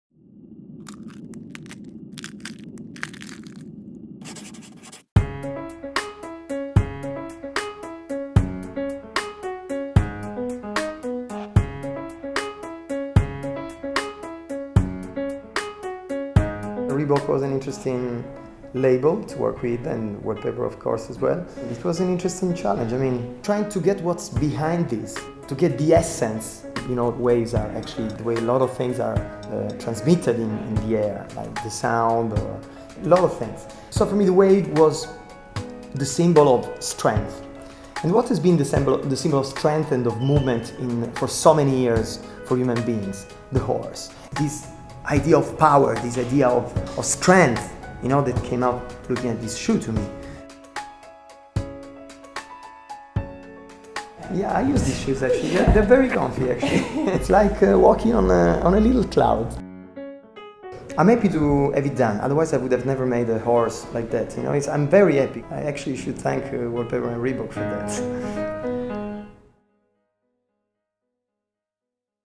Reebok x Wallpaper* Design Diaries -- Fabio Novembre
While impressed by the science of the ZigTech sole, Italian designer Fabio Novembre was more taken by its form, using this as his starting point for the creation of his life-size black horse sculpture. Made of soft polyurethane sprayed, using a new technique, with hard polyurethane, it is light enough to be lifted by two people. The perfecting of equine forms has proven a pivotal moment in art history, but 'movement and power has also long been connected to the horse -- we still speak of horsepower even in relation to motorbikes and cars,' Novembre says. 'So the horse became a strong motif for me.' The resulting artwork was, for the designer, also about the process and power of abstraction. Just as, he reckons, the designers of the ZigTech tried to abstract a sense of movement in the final form of the shoe's sole (quite aside from any performance benefit it brings), so he has sought to do so with idea of the horse, through the use of a wave form. 'Waves, of course, suggest the transmission of energy, through water, as sound and light,' Novembre adds. 'In fact, the literal translation of the Italian for a sea wave is "big horse". That's only just occurred to me.'